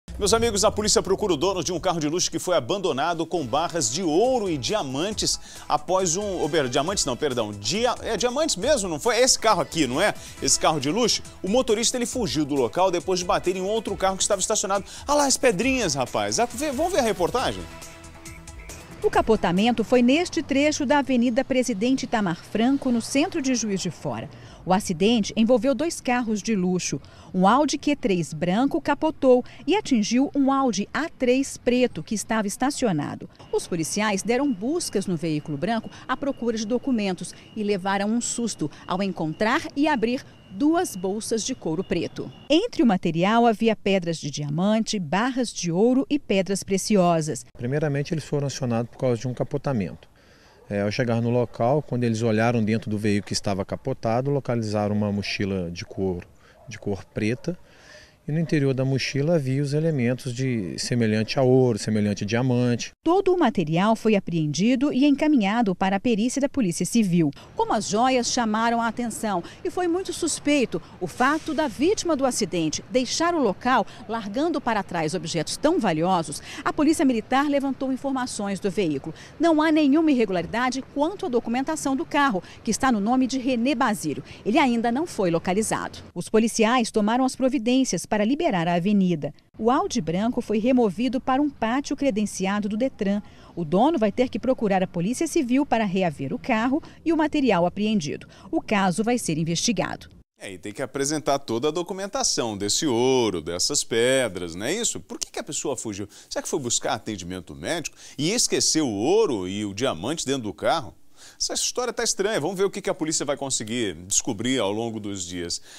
Motorista capota e abandona carro com joias e diamantes em Juiz de Fora (MG)
A polícia procura o dono de um carro de luxo que foi abandonado com barras de ouro e diamantes após capotar. O motorista fugiu do local depois de bater em outro veículo que estava estacionado.